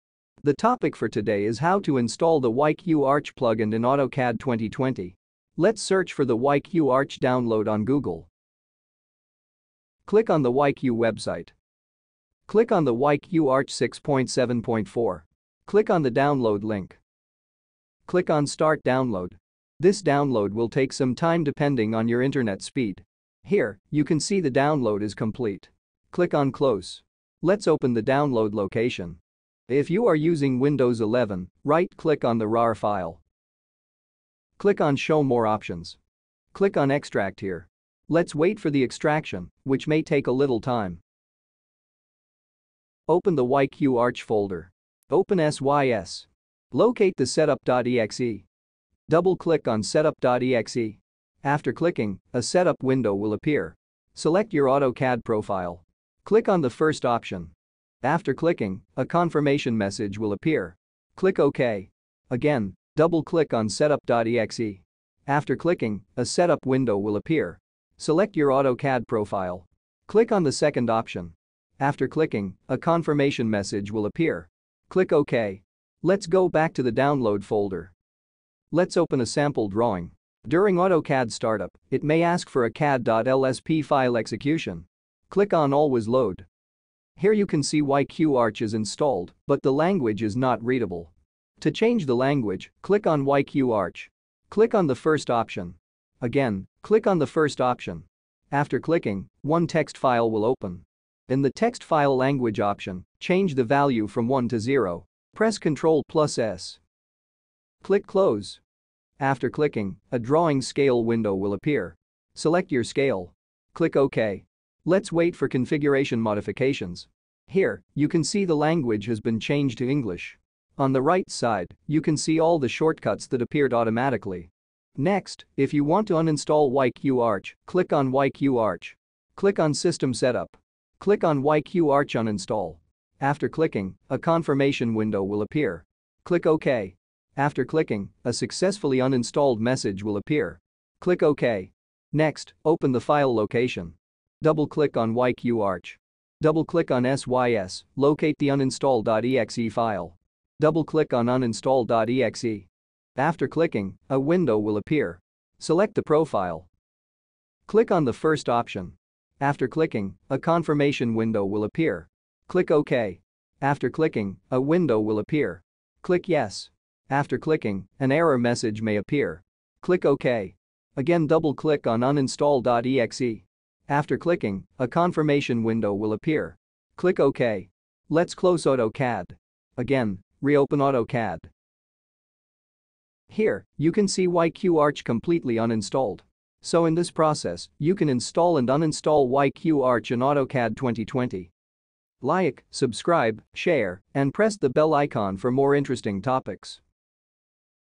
Install YQArch in AutoCAD 2020
Unleash the power of YQARCH in AutoCAD 2020 with our step-by-step installation guide! In this video, we walk you through the process of installing YQARCH to enhance your drafting and design capabilities. Say goodbye to complicated installations and hello to seamless integration. Upgrade your AutoCAD experience today.